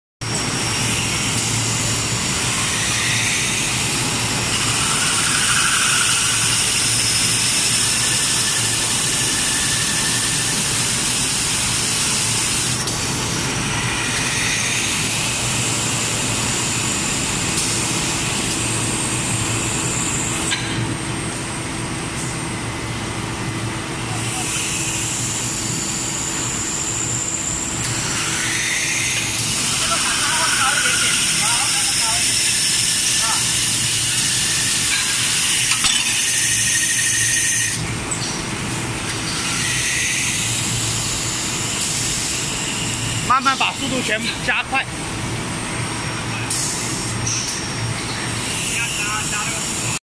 Stove glass/Kitchen glass drilling machine(big holes)--DED-02
Stove glass/Kitchen glass drilling machine(big holes), to dill the big holes of stove glass or kitchen glass;Drill two Holes one time .